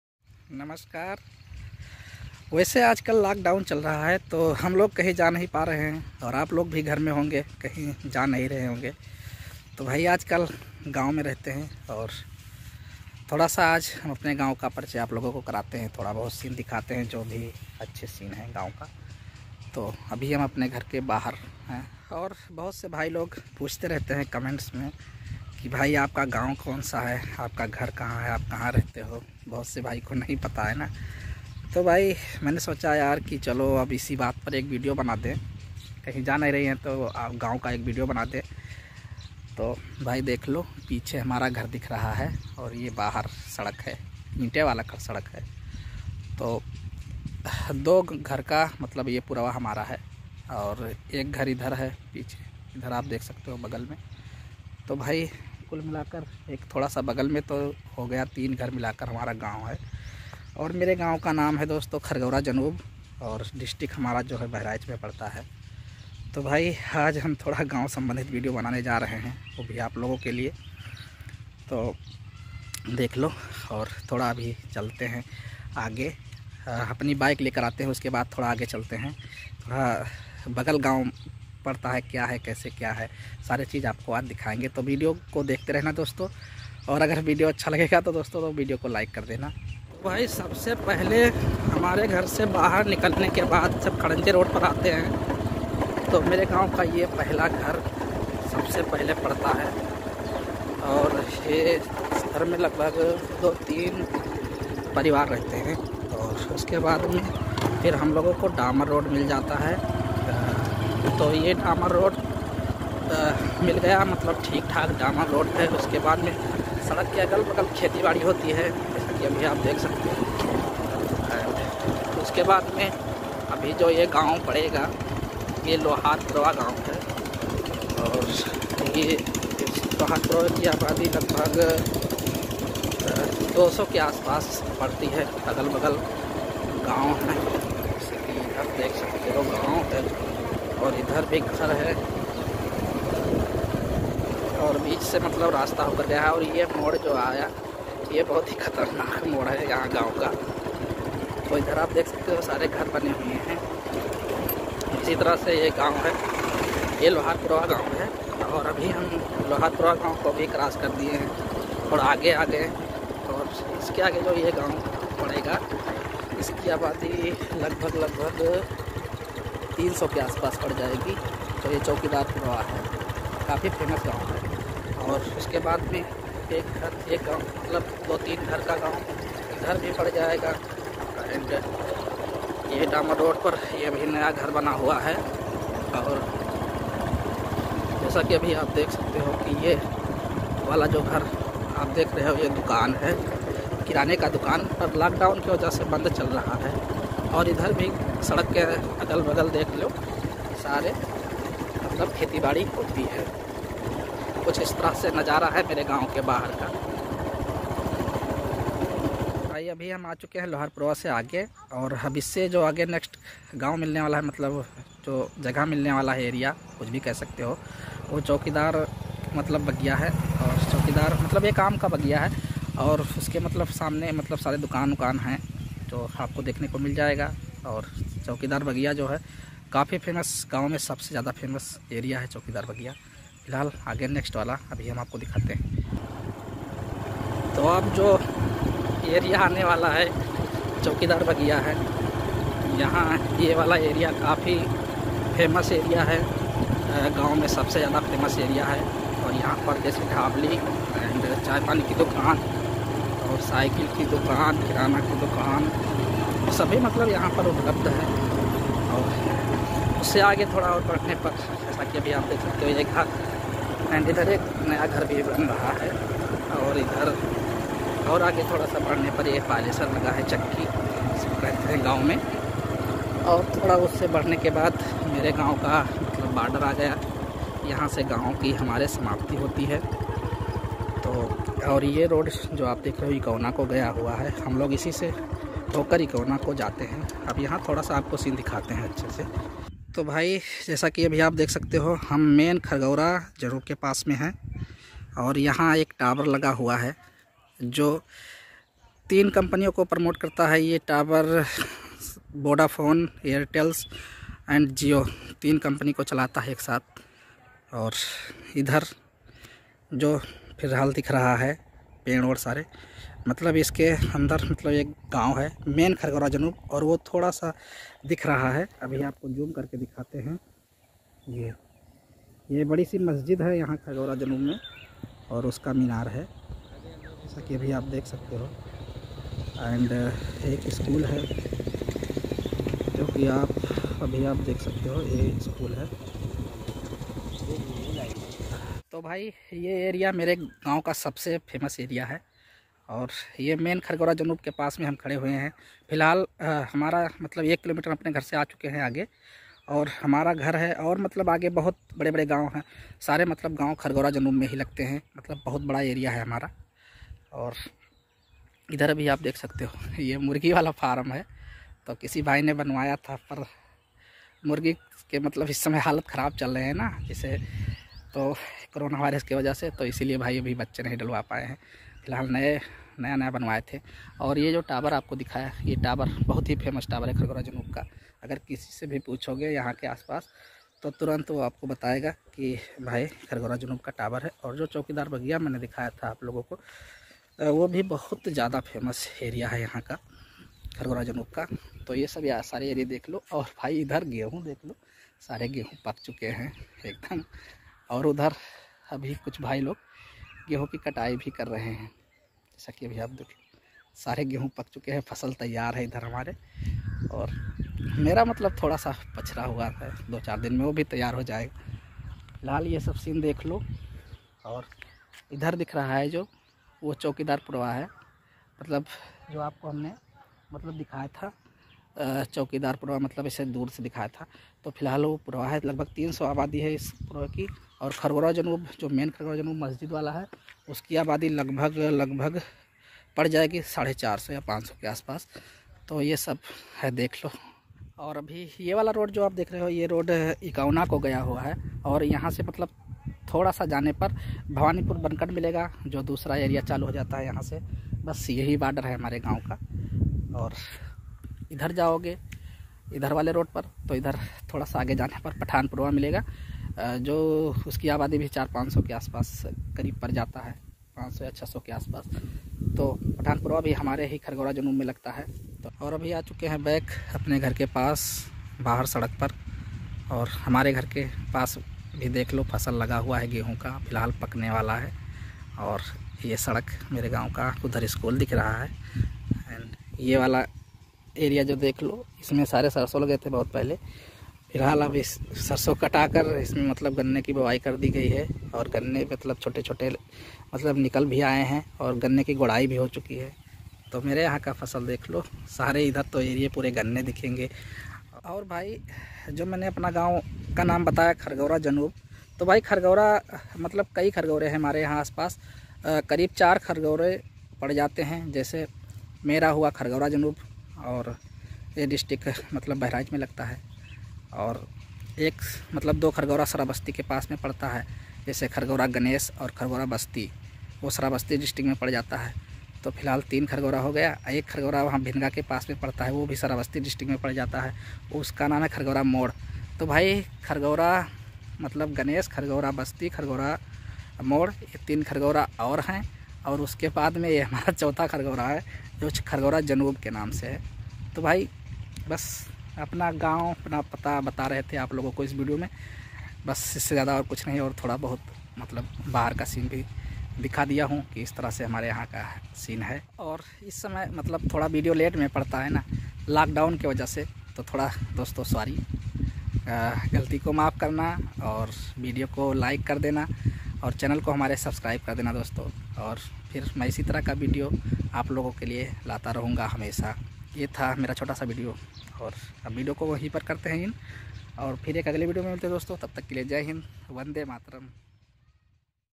मेरा गाँव | Mera Ganv
Hello friends

Mera name Brijesh kumar
Aur mere ganv ka name khargaura janoob
Distt Bahraich.

Dosto ye video mere ganv ka hai aur is video me ganv sambandhit hi seen dikhaye hai.

Dosto mai asha karta hoon ki mera ganv ap sabhi dosto ko jarur pasand aya hoga.

Dosto mai hamesha hi video Aplogo ke liye banata rahta hoon.